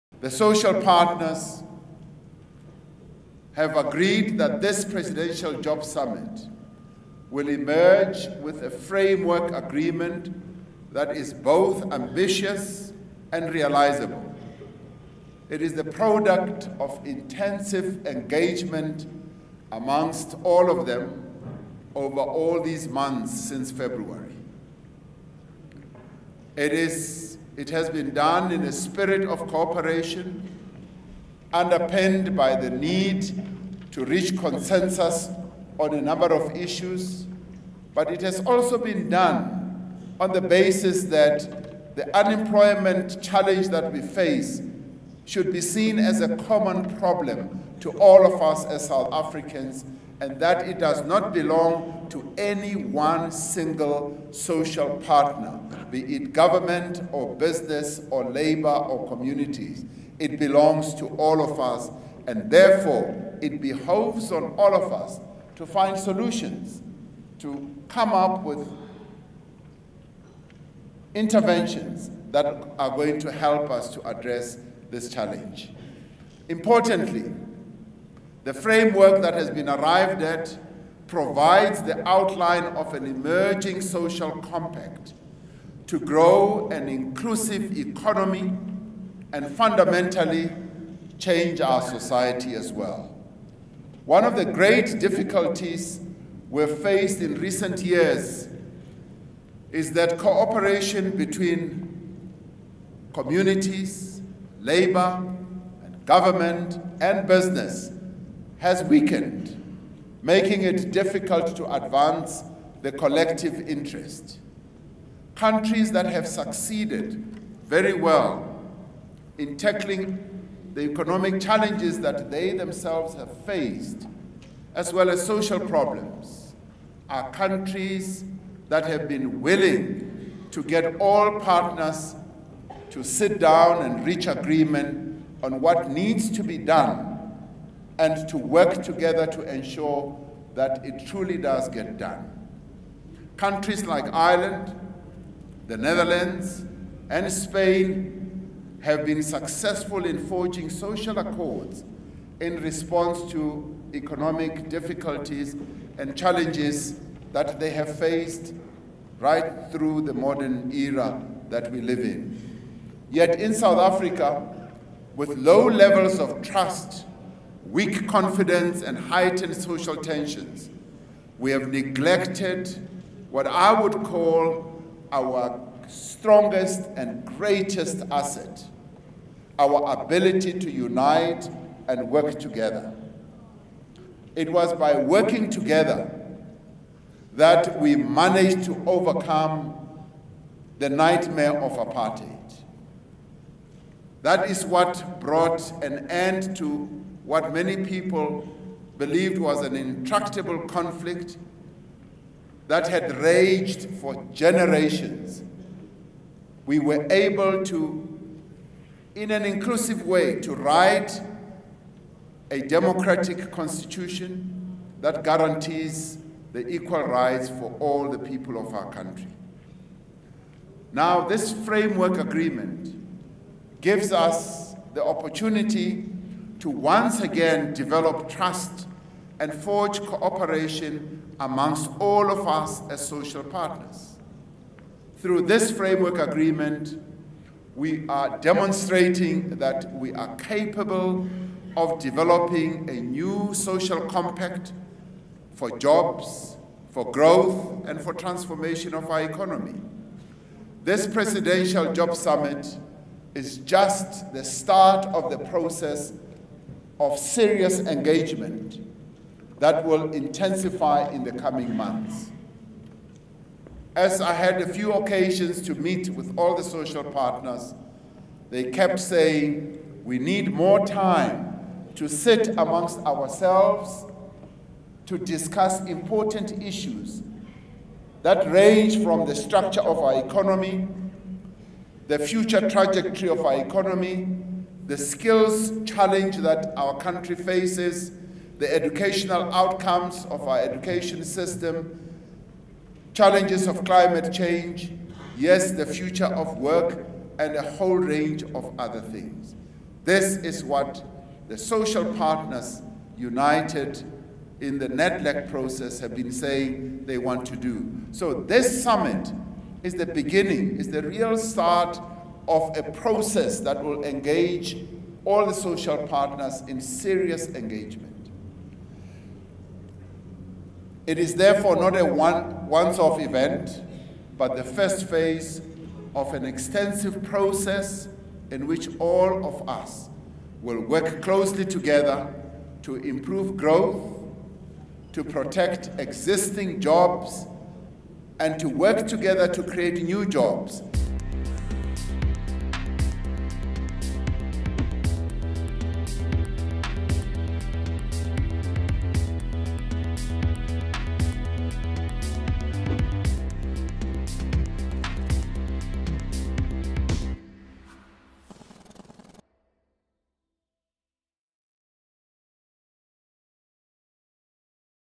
Detailed Jobs Summit package includes R100bn for black industrial firms
The detailed 84-page framework agreement unveiled by President Cyril Ramaphosa at the much-anticipated Jobs Summit on Thursday includes a reaffirmation of a commitment by the financial sector to invest R100-billion in black-owned industrial enterprises over the coming five years.